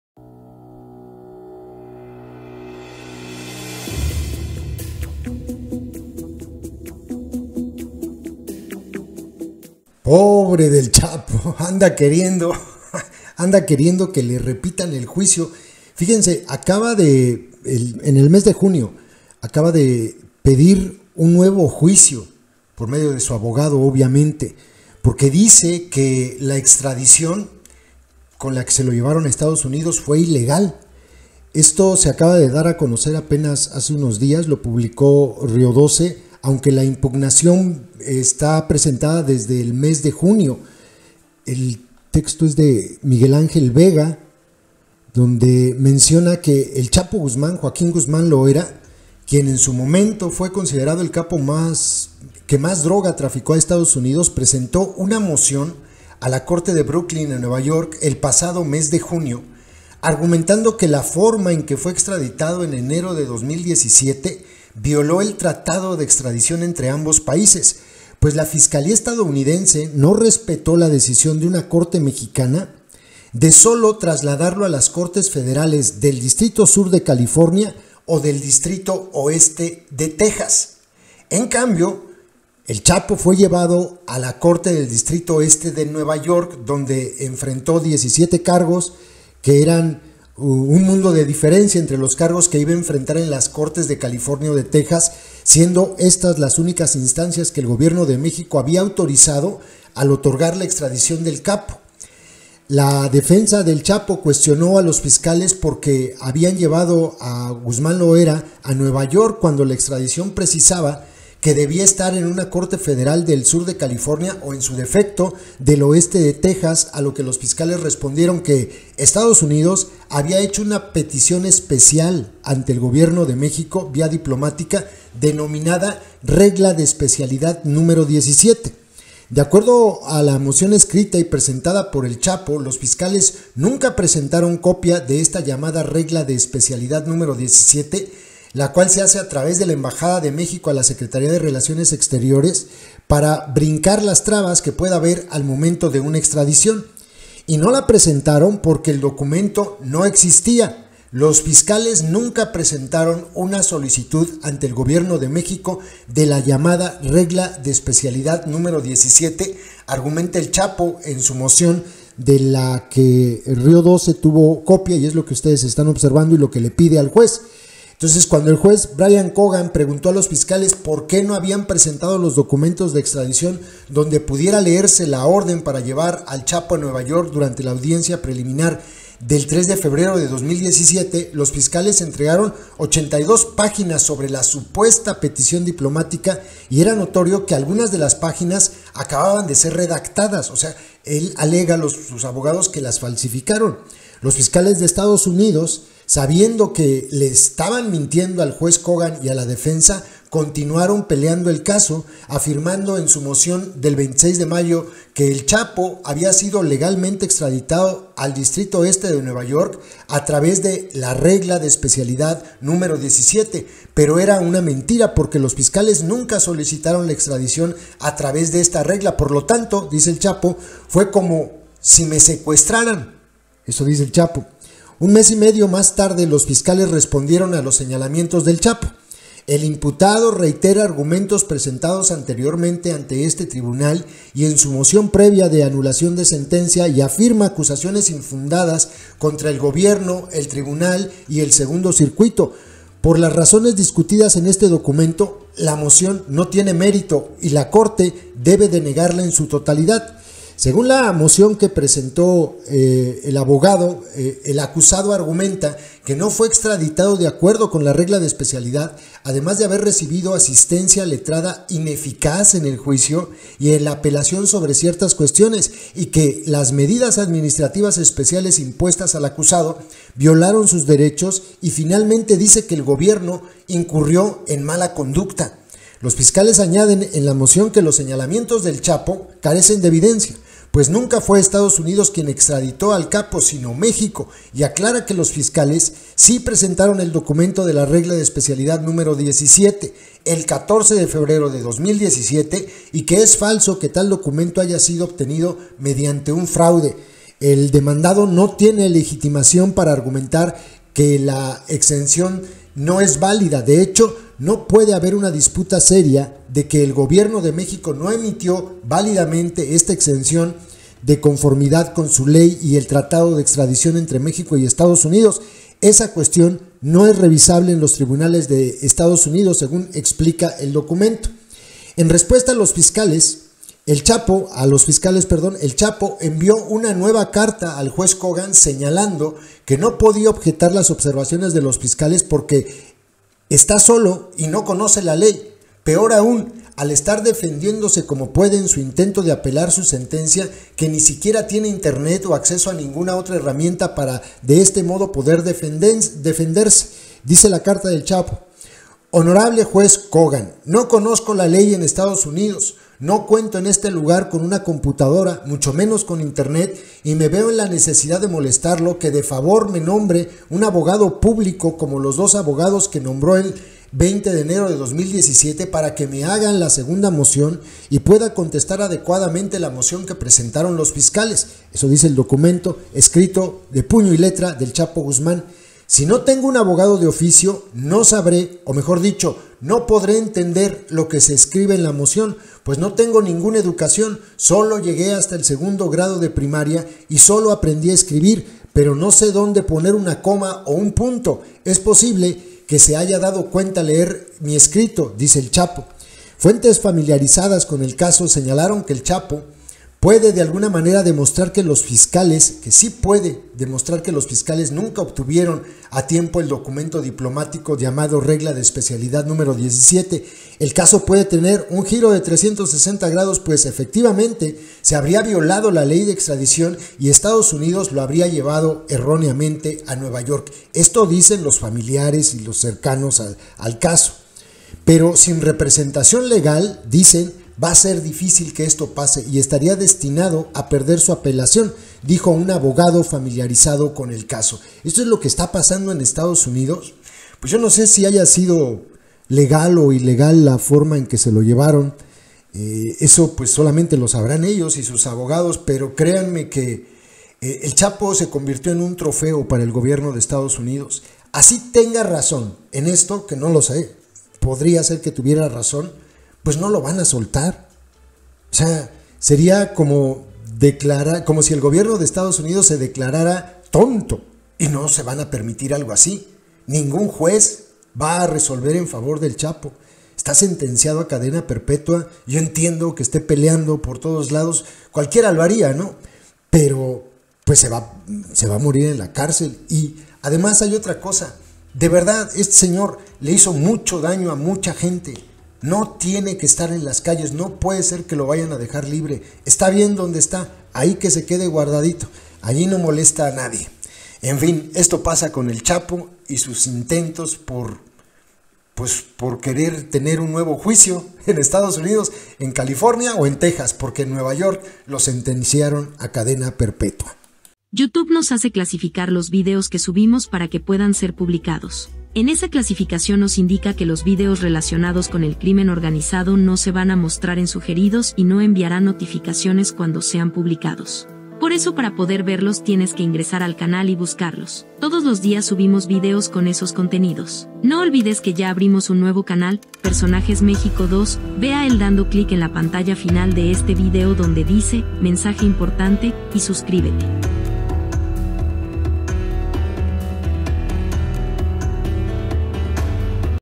El Chapo busca un nuevo juicio, porque dice que fue ilegal su extradición a Estados Unidos
Joaquín Guzmán Loera, El Chapo, presentó una moción a la corte de Brooklyn, Nueva York, el pasado mes de junio argumentando que la forma en que fue extraditado en enero de 2017 violó el tratado de extradición entre ambos países, pues la fiscalía estadounidense no respetó la decisión de una corte mexicana de sólo trasladarlo a las cortes federales del Distrito Sur de California o del Distrito Oeste de Texas.